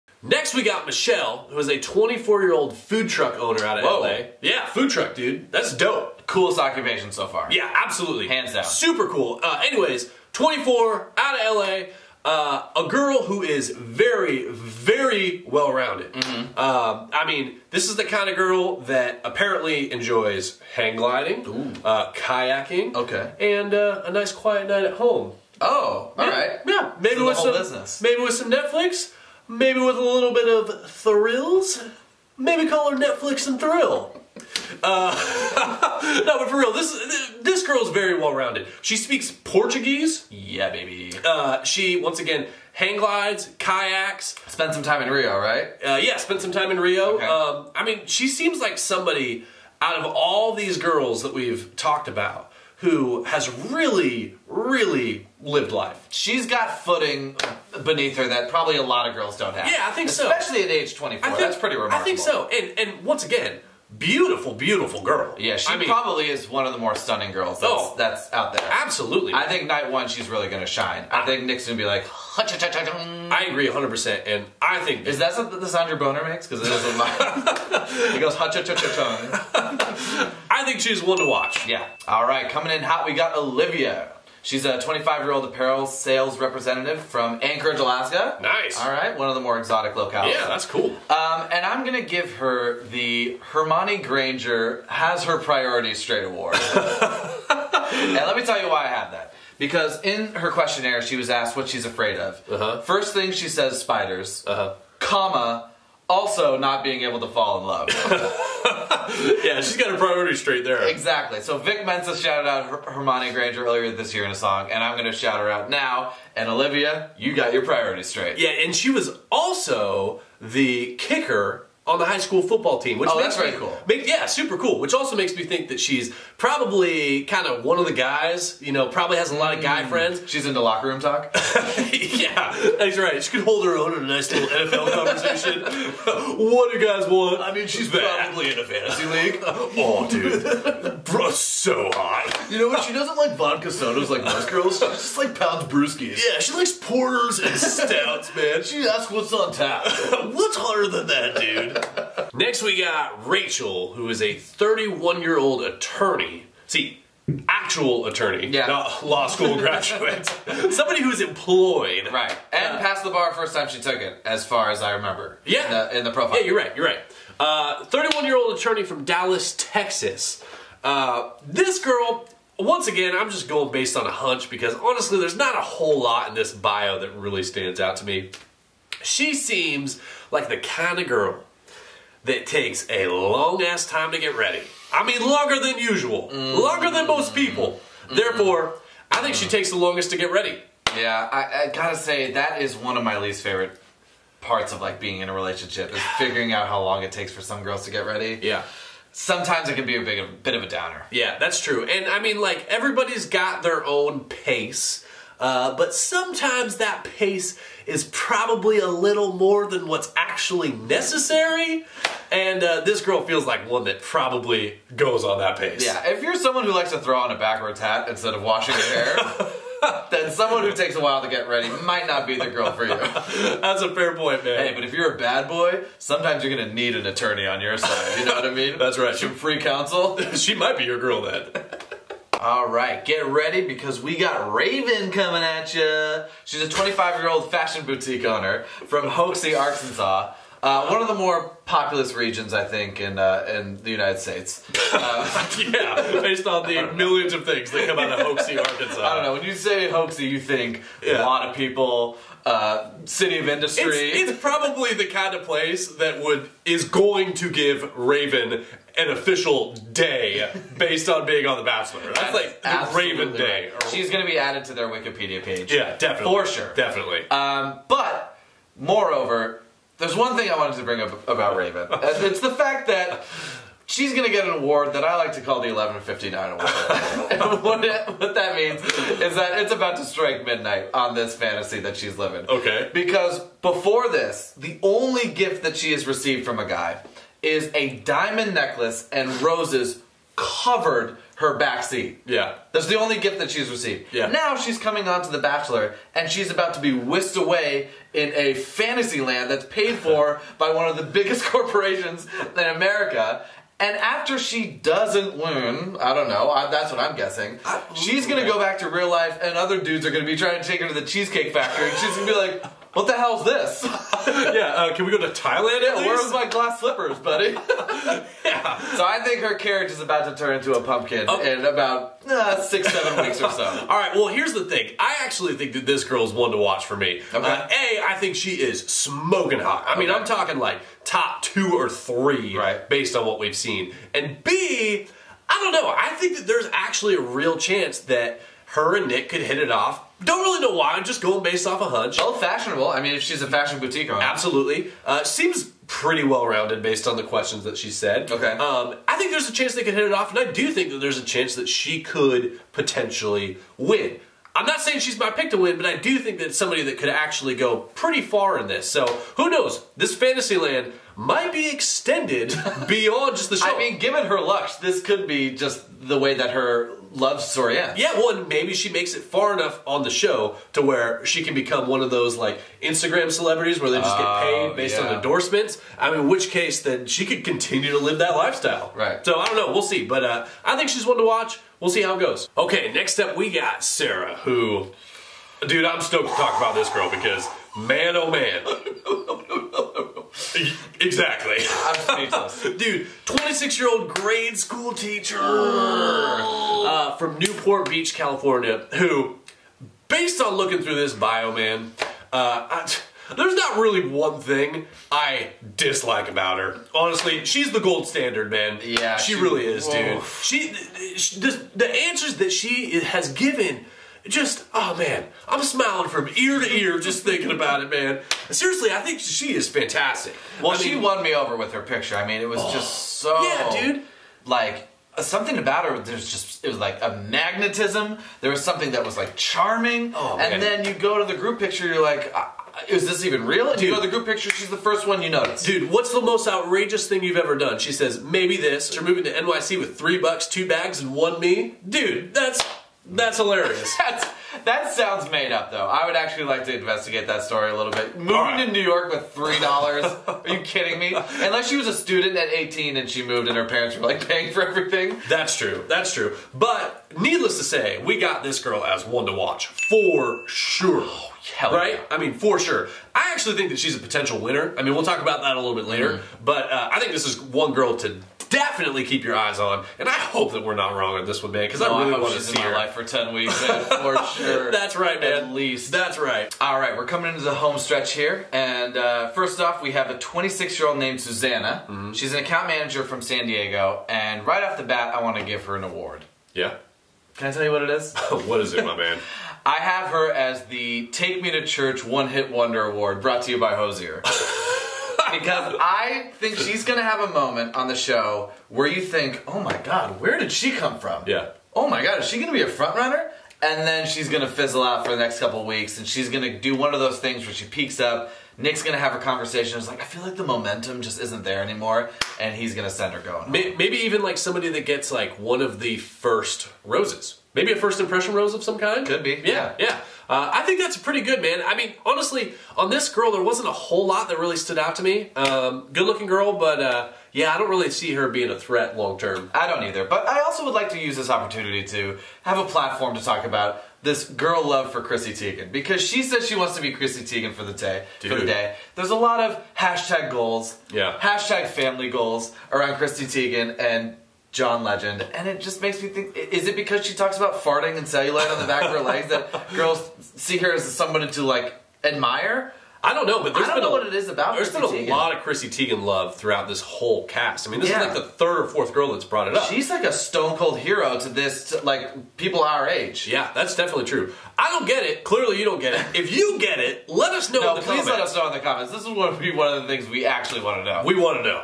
The Bachelor Season 21 | PREVIEW Extravaganza Part 4
The Bachelor Season 21. The Bachelor 2017. Nick Viall. Chris Harrison. ABC. Is this sure to be the most dramatic season in Bachelor history? We'll tell you in our annual PREVIEW Extravaganza! 🌹

💭 Did we get anyone wrong yet? Who are you feeling good about so far?

It's that time of year again. Join us as we break down each contestant on The Bachelor Season 21.

In this episode:
- Michelle (0:00) ⭐️ONE TO WATCH🏆
- Olivia (1:22)
- Rachel (2:32)
- Raven (3:54) ⭐️ONE TO WATCH?🤔
- Sarah (6:23) ⭐️ONE TO WATCH🏆
- Susannah (8:02)

As always, WE LOVE YA'LL!